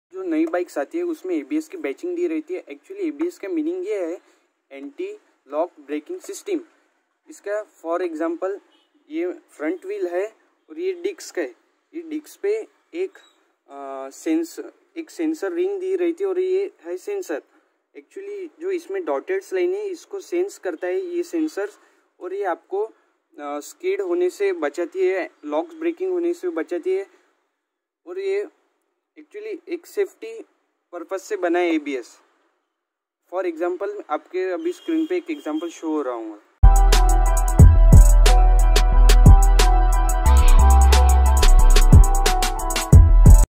ABS Explanation.
I explain shortly the basic concept of ABS working. please do share